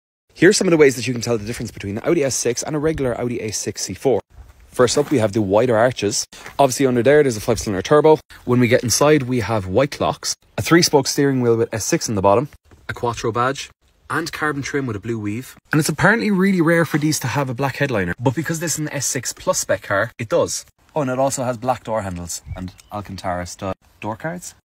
Did you know: the C4 generation was the first time Audi used the A6/S6 name, using it for the faceli
Did you know: the C4 generation was the first time Audi used the A6/S6 name, using it for the facelift of the C4 generation Audi 100/S4